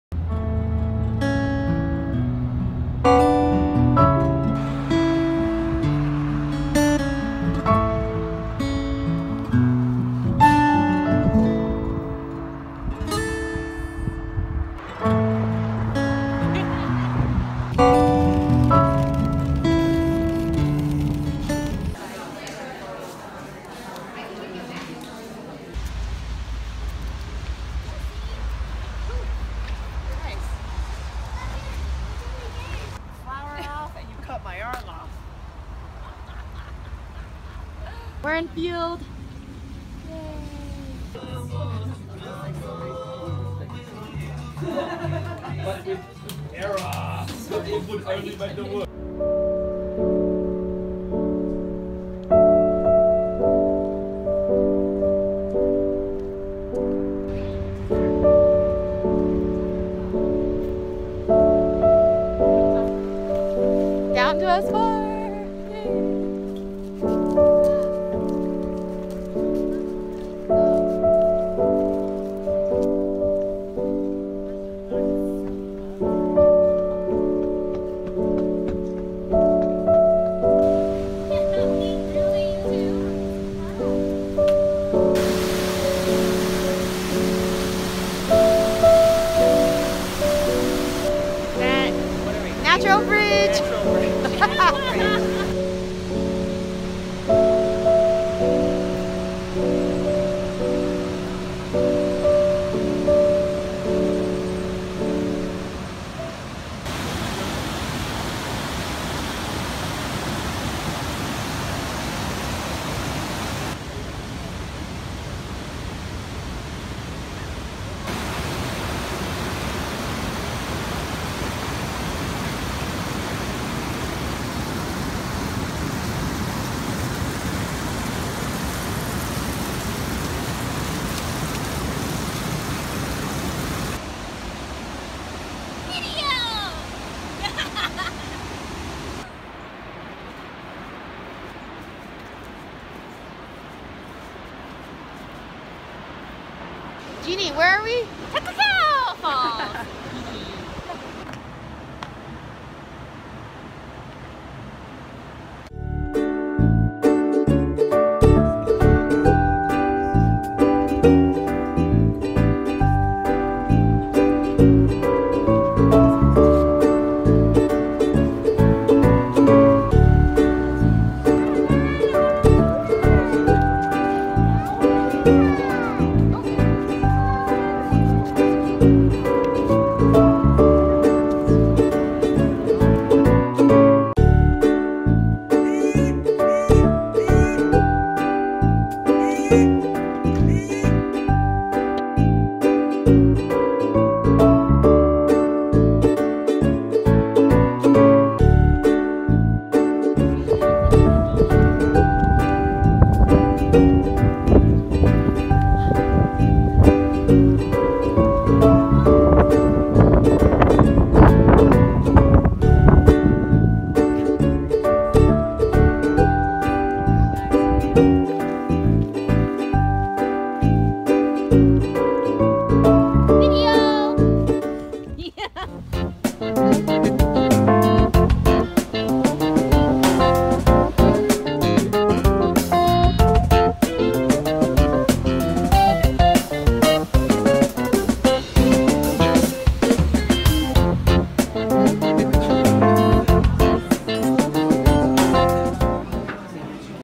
Banff August 2017 - Day 4
Yoho National Park - Emerald Lake, Natural Bridge, Takakkaw Falls and Banff - Moraine Lake